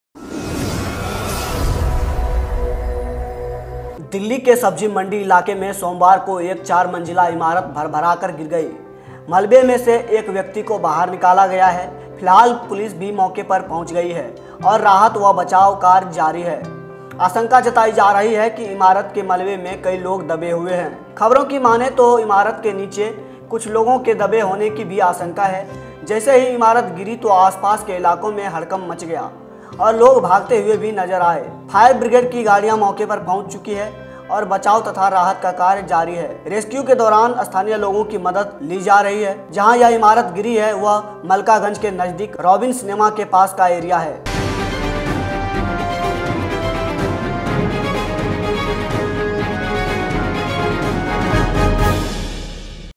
रॉबिन सिनेमा के पास दिल्ली के सब्जी मंडी इलाके में चार मंजिला इमारत गिरी, रेस्क्यू ऑपरेशन जारी
Welcome to Dilli Dastak.

Dilli Dastak is now.  Dailyhunt & Public App